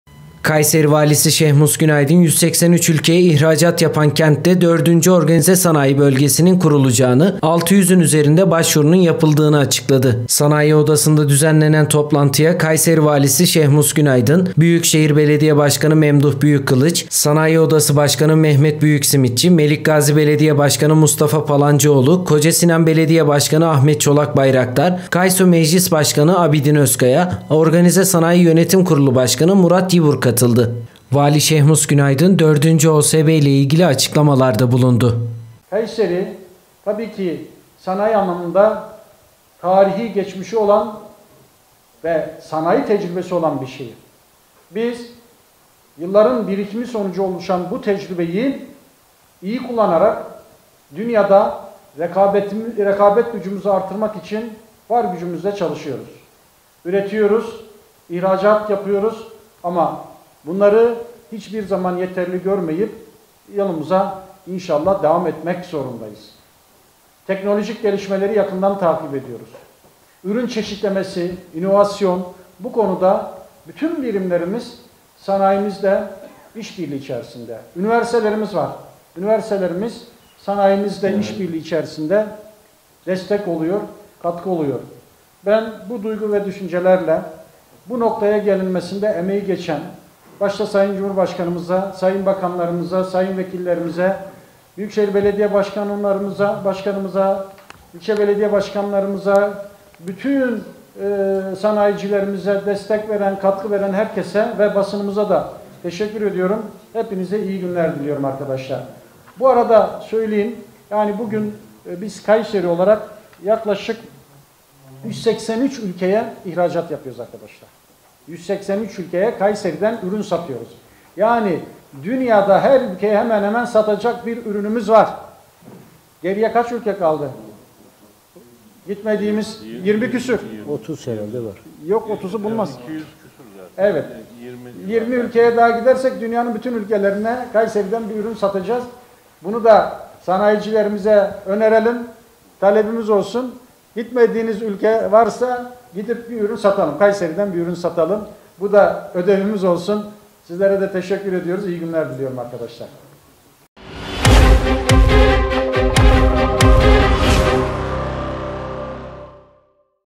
183 Ülkeye ihracat yapan Kayseri'ye 4'ncü OSB kuruluyor
KAYSERİ,(BGC) - Kayseri Valisi Şehmus Günaydın, 183 ülkeye ihracat yapan kentte 4'ncü Organize Sanayi Bölgesinin kurulacağını açıkladı. 600'ün üzerine başvurunun yapıldığını açıkladı.
Sanayi Odasına düzenlenen toplantıya Kayseri Valisi Şehmus Günaydın, Büyükşehir Belediye Başkanı Memduh Büyükkılıç, Sanayi Odası Başkanı Mehmet Büyüksimitçi, Melikgazi Belediye Başkanı Mustafa Palancıoğlu, Kocasinan Belediye Başkanı Ahmet Çolakbayrakdar, KAYSO Meclis Başkanı Abidin Özkaya, Organize Sanayi Bölge Yönetim Kurulu Başkanı Murat Yibur katıldı.
Kayseri, ticari anlamında önemli merkez olduğunu söyleyen Vali Şehmus Günaydın, OSB, Serbest Bölge, İncesu OSB ve Mimarsinan OSB üretilen ürünlerin 183 ülkeye ihraç edildiğini ifade etti. Kayseri'de üretim hacminin her geçen gün artığının yapılan fabrikalarla 10 bin yeni istihdamın oluştuğuna dikkat çeken Vali Şehmus Günaydın, " Kayseri'de yatırımın, üretim hacminin ve istihdamın artmasına yönelik önemli adımlar atıyoruz. Kayseri'de bilindiği üzere 3 Organize Sanayi Bölgemiz var. Burada ihracat rakamları pandemi dönemin henüz bitmemesine rağmen ciddi bir artış içerisinde. Hedeflediğimiz rakamlara yıl sonu itibariyle ulaşmış olacağımızı ümit ediyorum. Kayseri kurulması için yola çıktığımız 4'ncü OSB  Sivas yolunda, Geri Gönderme merkezimiz arkasında olacak. Şu an için mevcut farklı alanlardan birçok firma yatırım için yer istiyor. Alan olarak 14 milyon metrekare alan için talep var. Ancak biz ilk etapta 5 milyon metrekaresini karşılayabileceğiz.  Bu işe hızla eğilim göstereceğiz. Kayseri 183 ülkeye ihraç yapan bir şehir. Geriye 20 ülke kaldı. Ödevimiz bu 20 ülkeye ihracat yapmak" diye konuştu.